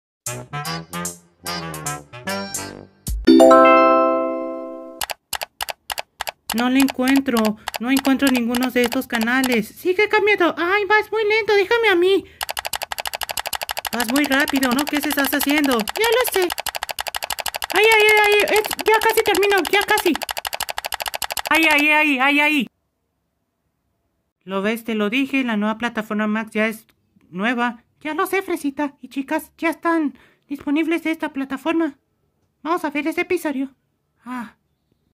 Elmo's World Reacts! Toons: Toon Fast por Max
Elmo y sus 4 amigas estaban viendo la nueva plataforma Max (Antes HBO Max).